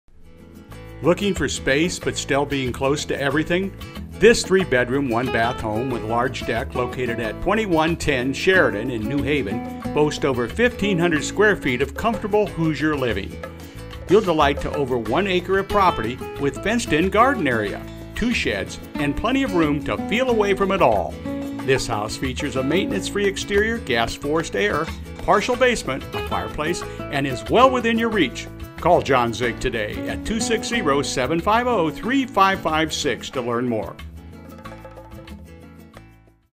John Zieg's Listing at 2110 Sheridan Road in Fort Wayne, Indiana.mpg
Looking for space but still being close to everything?
This 3 bedroom 1 bath home with large deck, located at 2110 Sheridan in New Haven boasts over 1500 square feet of comfortable Hoosier living.
You'll delight to over 1 acre of property with fenced in garden, 2 sheds and plenty of room to feel away from it all.
The home features a maintenance free exterior, gas forced air, partial basement, a fire place and is well within your reach.